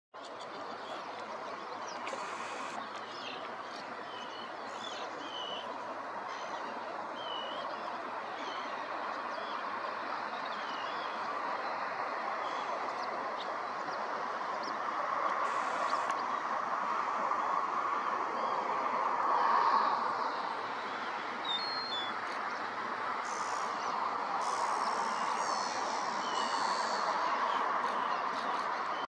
Kelp Gull (also called Cape gull) - Cambridgeshire - 09/08/2022
Cape Gull is a South African sub species of Kelp gull, if accepted by BBRC and BOURC, will be the 1st record for the UK.